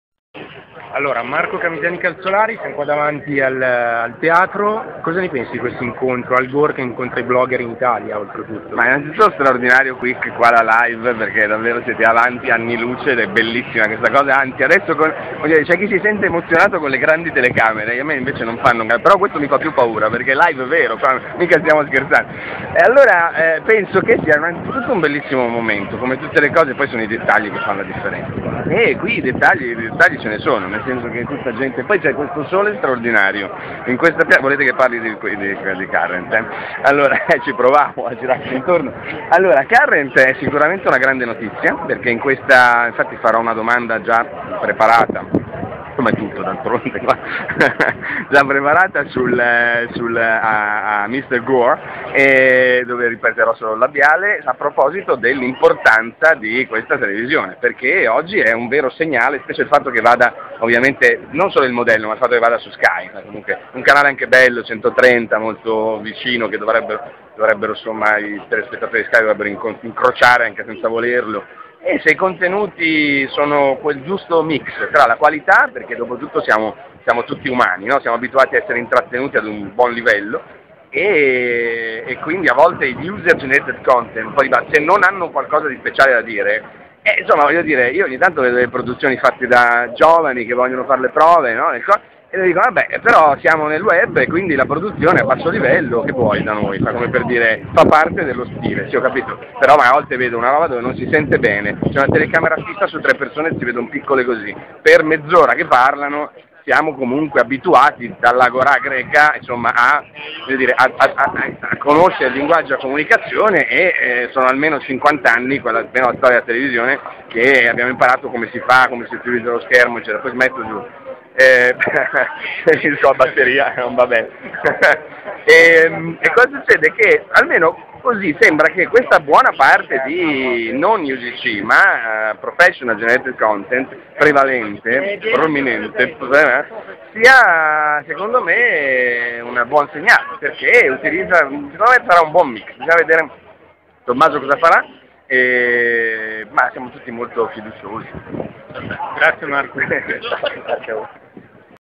Current Tv Blogosfere intervista Marco Camisani Calzolari 2
Al Gore incontra i blogger a Roma. Blogosfere ne parla con Marco Camisani Calzolari di Matteo Failla.